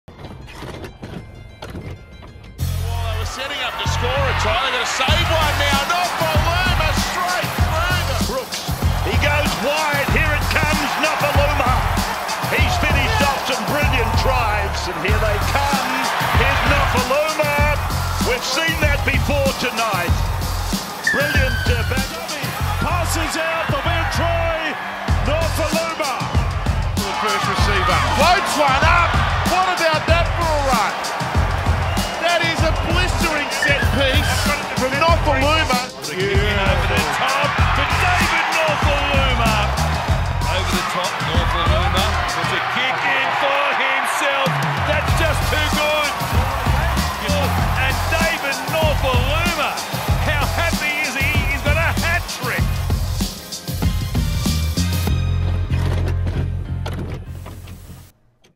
The Best of David Nofoaluma in 2019
Wests Tigers winger David Nofoaluma became just the fifth player in history to reach 50 tries for the club as he again took several strides forward in his game throughout the 2019 season.